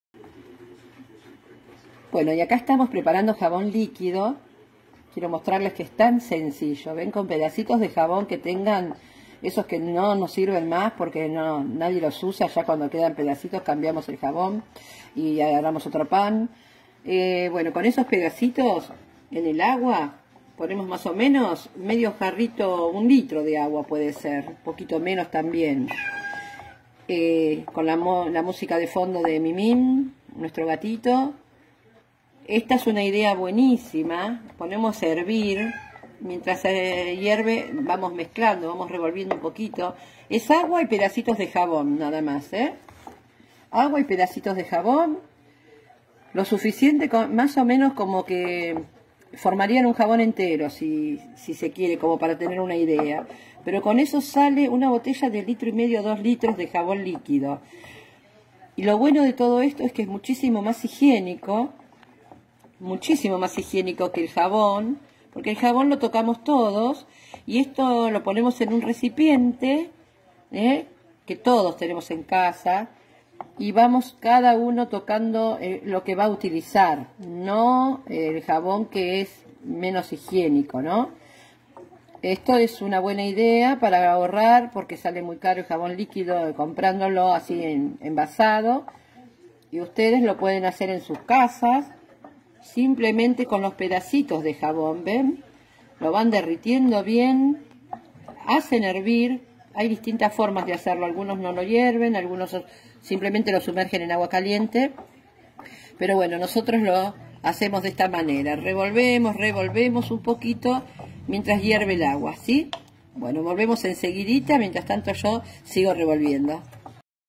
COMO HACER JABON LIQUIDO FACIL Y ECONOMICO
En este video te enseñamos a tener jabón líquido